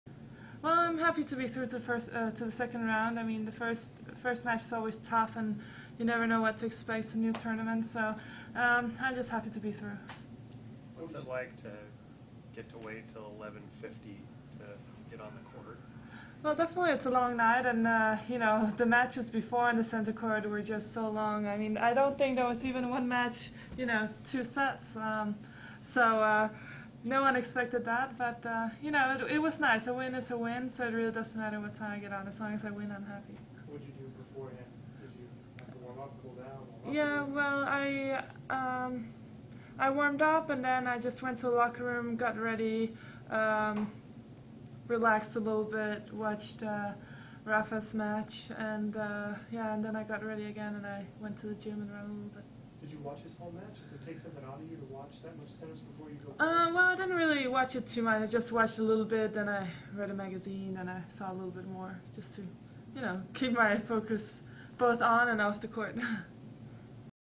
2010 US Open Press Conferences: Caroline Wozniacki (First Round)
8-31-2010

Caroline Wozniacki (DEN) d. Chelsey Gullickson (USA)

WATCH THE 2010 US OPEN!

Broadcasted also on CBS, ESPN2 & The Tennis Channel